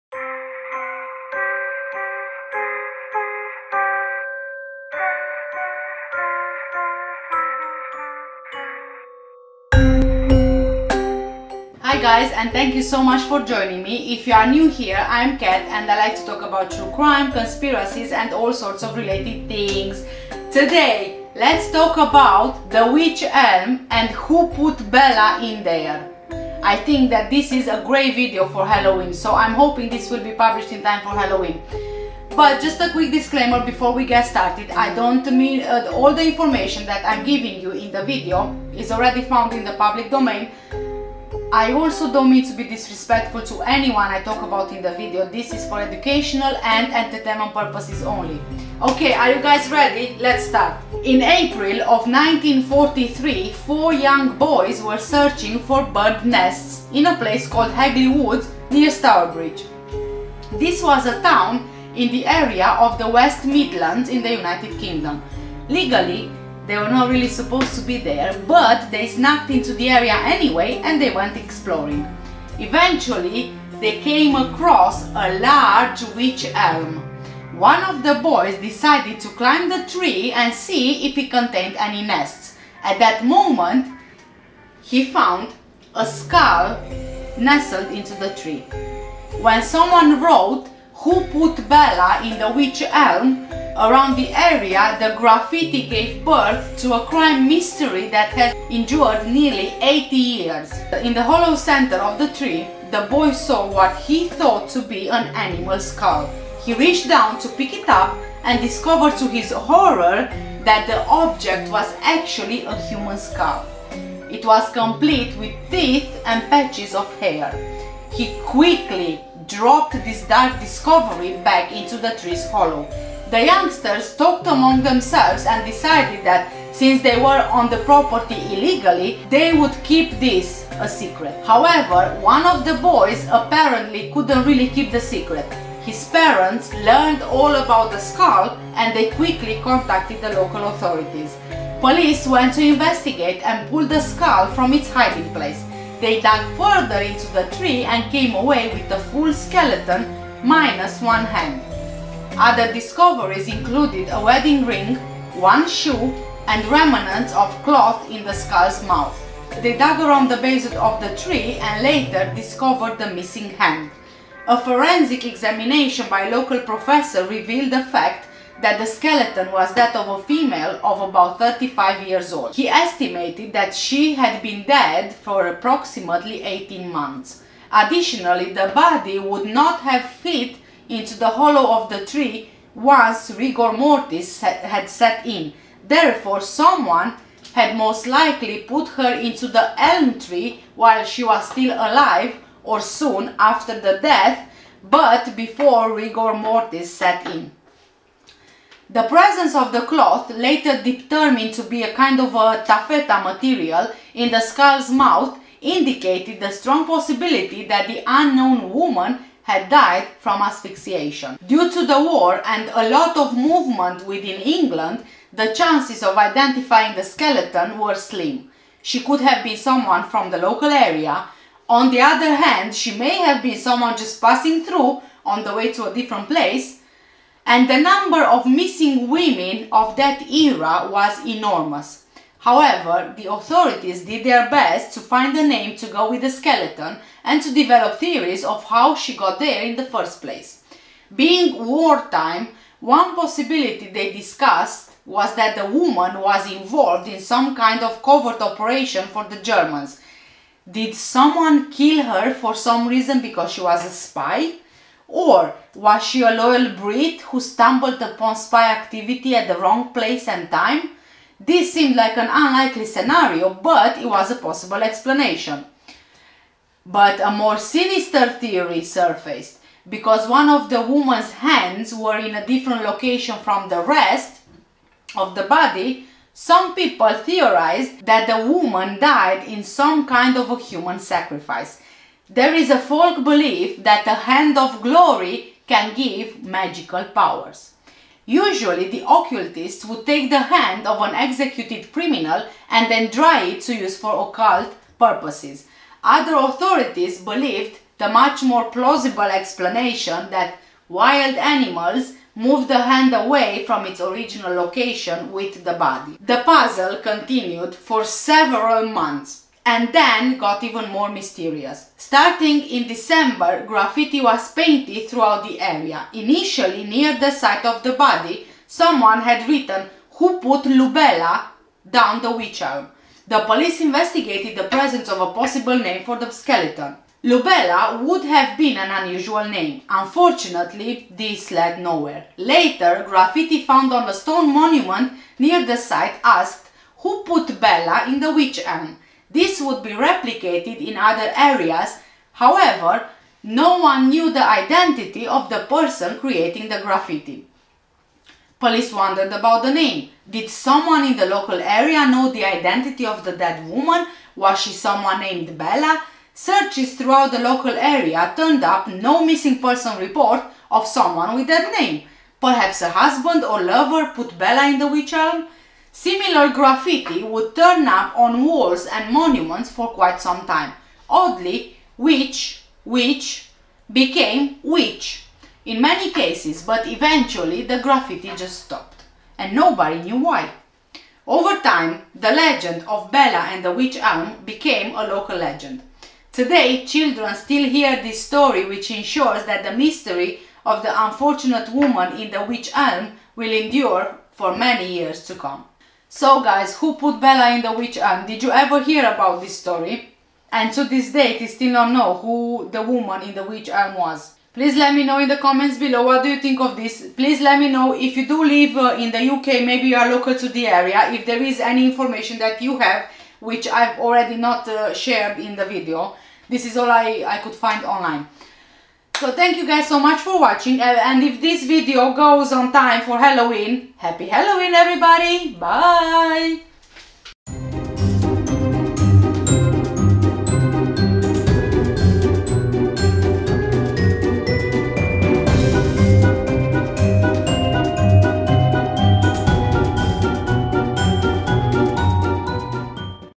WHO PUT BELLA IN THE WYCH ELM?
In April of 1943, four young boys were searching for bird nests in a place called Hagley Woods near Stourbridge. This was a town in the area of the West Midlands, UK. Legally, they were not supposed to be there, but they snuck into the area and went exploring.
Eventually, they came across a large Wych elm. One of the boys decided to climb the tree and see if it contained any nests. At that moment he found a skull nestled in the tree. When someone wrote “Who put Bella in the Wych Elm?” around the area, the graffiti gave birth to a crime mystery that has endured nearly 80 years.